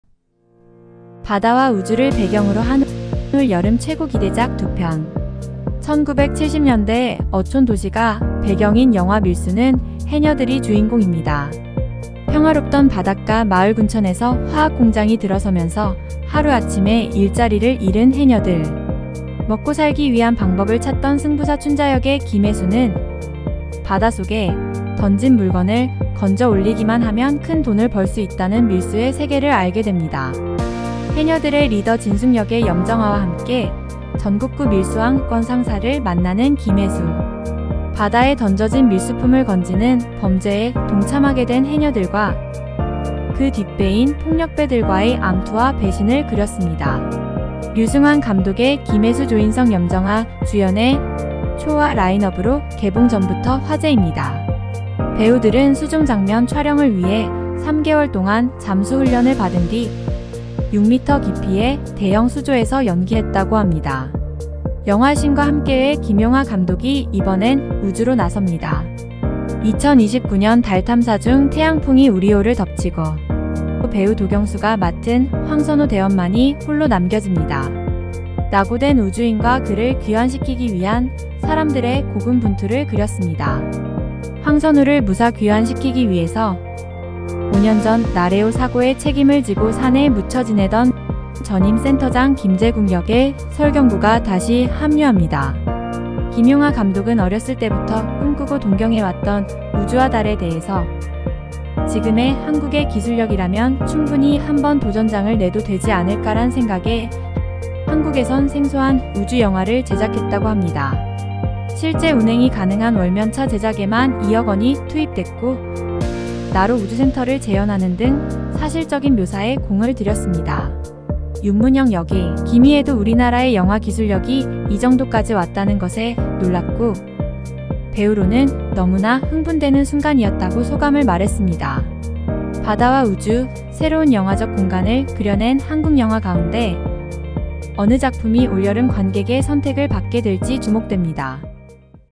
올 여름 개봉 영화 최고 기대작 두편, 여러분의 선택은?
올 여름 한국 영화로는 생소한 소재를 다룬 밀수와 더문, 영화 두편이 개봉합니다. 한 편은 바다를 배경으로 한편은 우주를 배경으로 하는 블록버스터 한국영화, 여러분은 어느 쪽이 더 재밌다고 보십니까? 물론 둘다 재밌겠죠^^